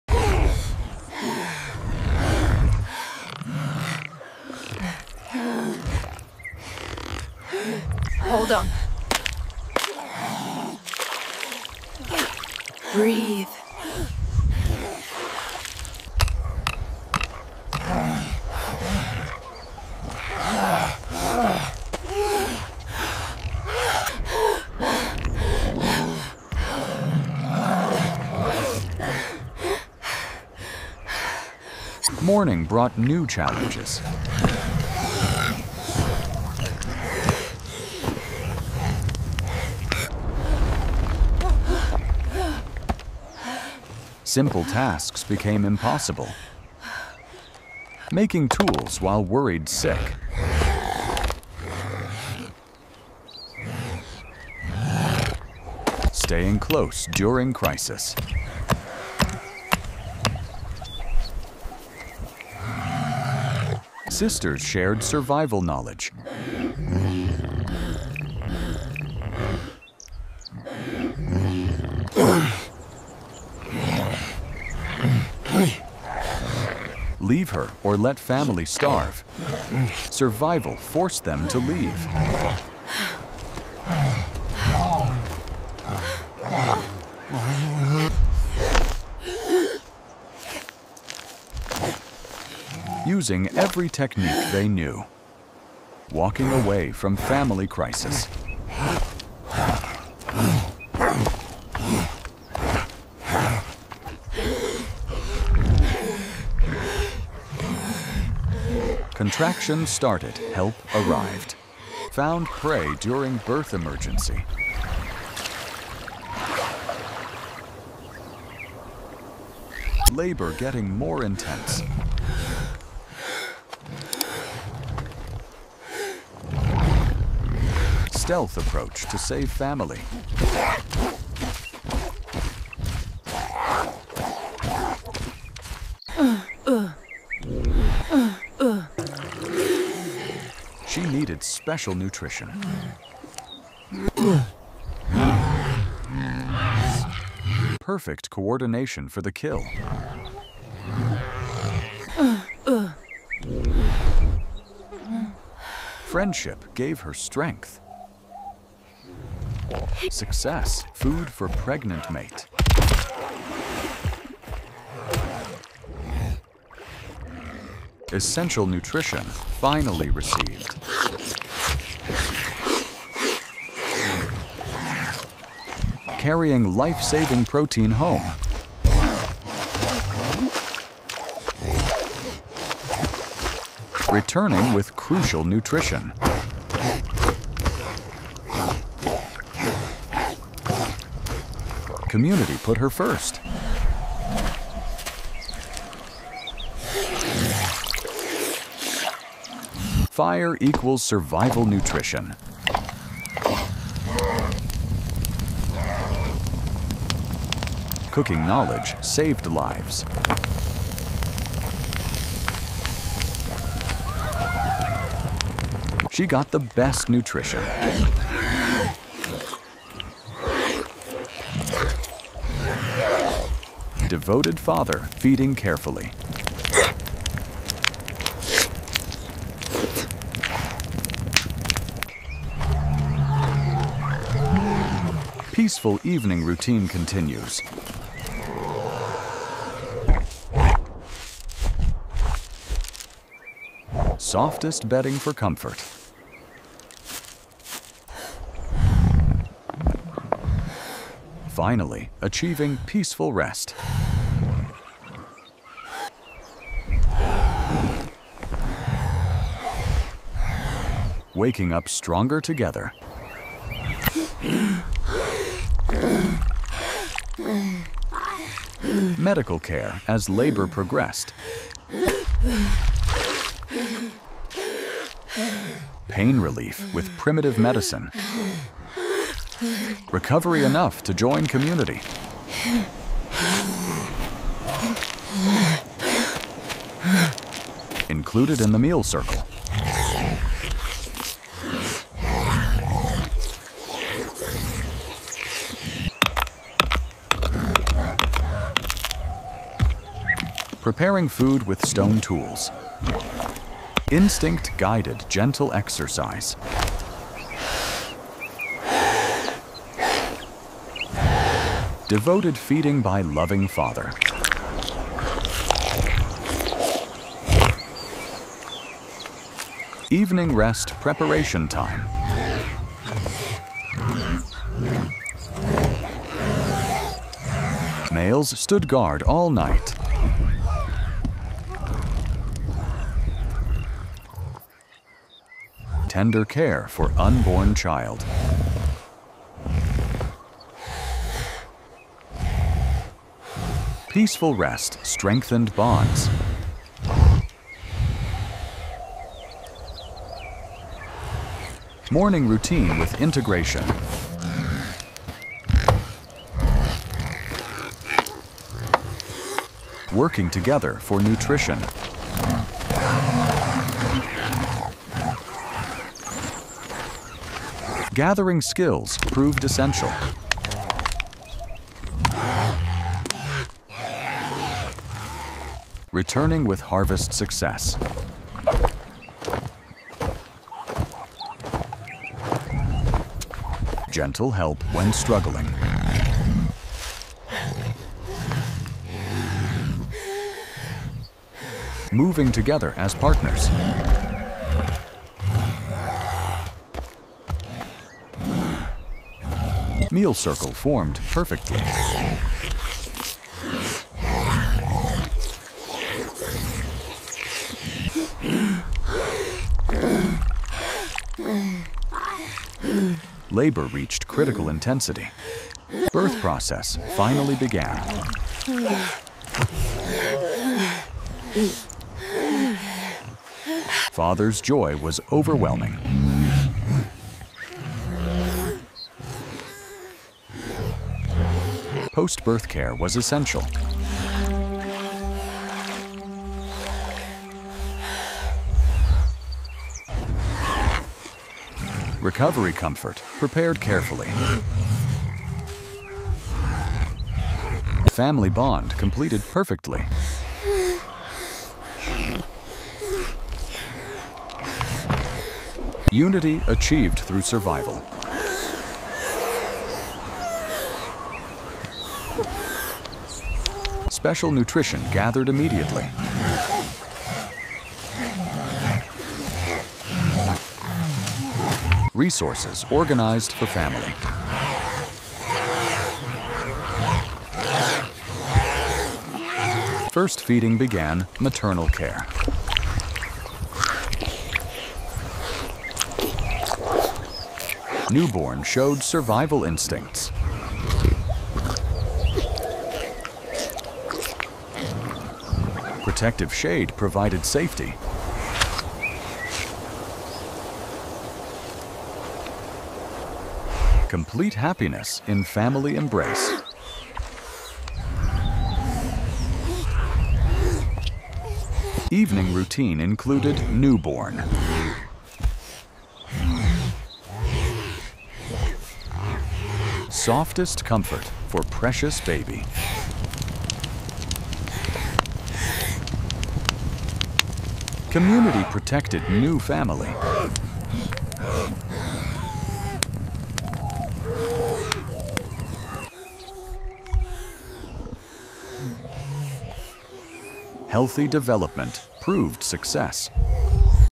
Life 5.5 Million Years Ago | Ancient Birth Survival and Family Bonds
Five point five million years ago, ancient family bonds and community cooperation faced their ultimate test during birth survival in the harsh African environment. This incredible reconstruction reveals how primitive medical knowledge, maternal instincts, and family protection created humanity's most crucial survival milestone - ensuring the next generation through successful birth and family care.
The Ancient Family Survival Journey:
Pregnancy Support Crisis - Community mothers share primitive knowledge to help struggling pregnant member survive daily challenges
Family Decision Conflict - Males torn between hunting necessity and staying to protect vulnerable pregnant mate during critical time
Primitive Birth Medicine - Female cooperation using leaves, water, and instinctive healing methods to ensure successful delivery
Community Birth Support - Ancient family bonds strengthen as entire community rallies around vulnerable mother during labor crisis
Successful Family Formation - Miracle of birth creates new family unit through ancient determination and community medical assistance
Post-Birth Family Care - Primitive knowledge ensures mother and infant recovery through devoted family attention and protection
Maternal Bonding Success - First feeding techniques demonstrate powerful maternal instincts and early family bond formation
Family Protection Network - Community vigilance guards vulnerable new family through dangerous nights with spear-wielding guards
Father-Child Connection - Paternal bonding shows early family relationship development and protective instincts in action
Family Milestone Celebration - Community celebrates successful birth survival as triumph of ancient family cooperation and bonds
Watch as 5.5 million year old human ancestors navigated family formation during prehistory's harshest conditions. From community birth support to family meal sharing, witness how primitive family bonds, maternal devotion, and community cooperation created the foundation for human family survival.
Most Incredible Family Survival Moments:
Community rally around pregnant member showing ancient family support systems in action
Birth survival through primitive medical knowledge and female cooperation during crisis
Father's overwhelming joy and protective instincts during first moments with newborn child
Community meal sharing prioritizing new mother's nutrition needs through family resource management
Family bedding preparation showing devoted care and comfort provision for vulnerable members
Protective night watch proving community commitment to defending vulnerable family units
Maternal feeding techniques demonstrating powerful family bonding and survival instincts
Family celebration of survival success showing community pride in successful birth outcome
This documentary reveals how 5.5 million years ago, family bonds, community cooperation, and protective instincts created humanity's most important achievement - successful family formation and survival through birth and early childcare.
The Stakes: Support family members or risk losing the next generation. Provide community protection or face family extinction. Ensure successful birth or lose genetic legacy. Every decision determined whether ancient families could survive and thrive.
Historical Accuracy: Based on archaeological evidence of early human family structures, community birthing practices, and ancient childcare cooperation. All character features reflect accurate 5.5 million year old Australopithecus family characteristics and harsh prehistoric survival conditions.
Epic Family Adventure: 68 segments of family survival drama, 9 minutes of continuous family triumph, documentary-quality reconstruction of humanity's greatest family achievement - successful birth survival and family bond formation.
Like if this ancient family journey moved you
Comment: How do you think family bonds helped early humans survive?